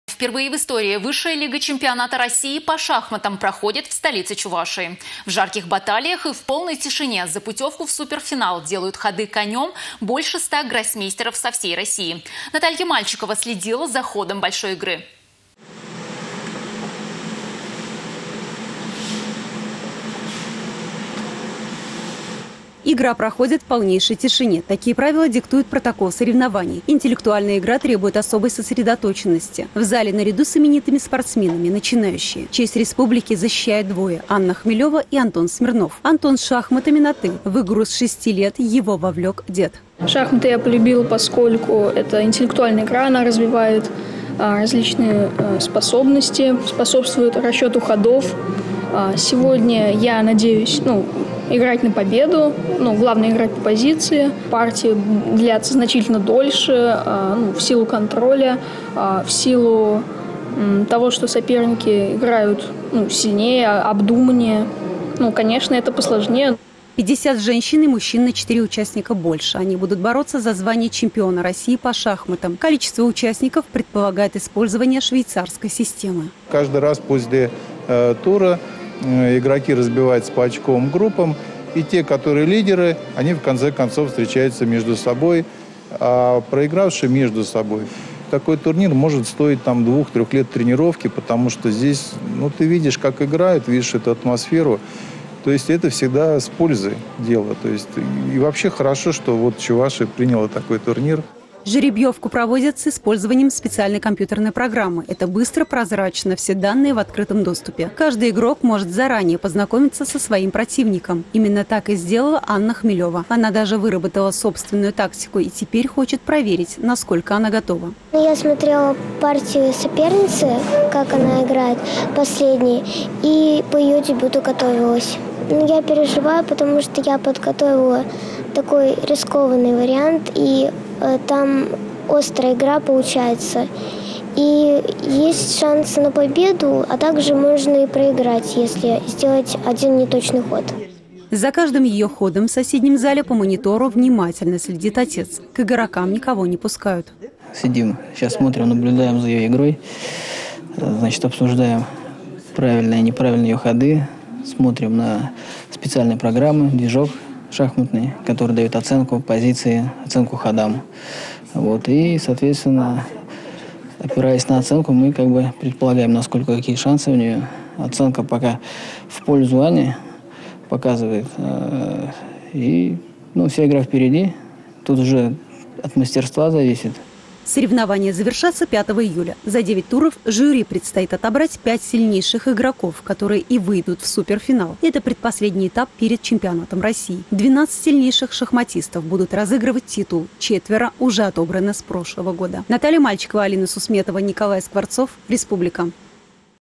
В Чебоксарах стартовала высшая лига чемпионата России по шахматам
Впервые в истории Высшая лига чемпионата России по шахматам проходит в столице Чувашии. В жарких баталиях и в полной тишине за путевку в суперфинал делают ходЫ конем больше ста гроссмейстеров со всей России. Наталья Мальчикова за ходом большой игры.